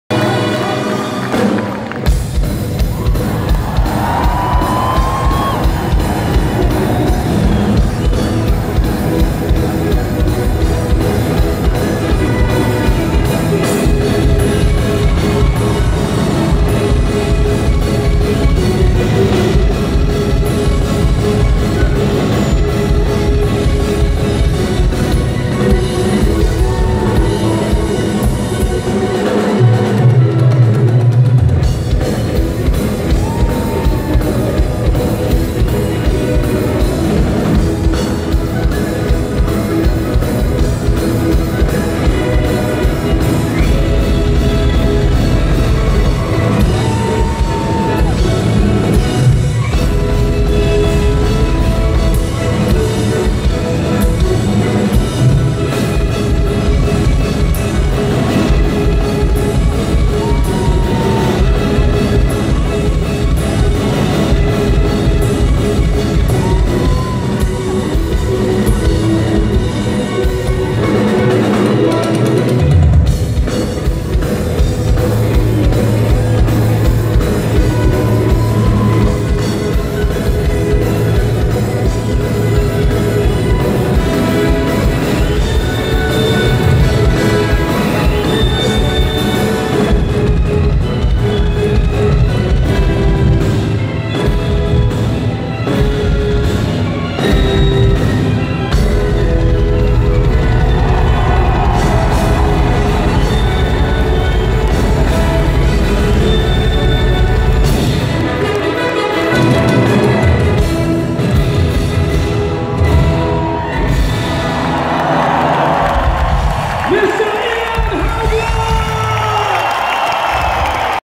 Europe - Drum Solo (Ian Haugland) (live chile 2017)
Europe War of Kings Tour en Chile 30 de mayo, 2017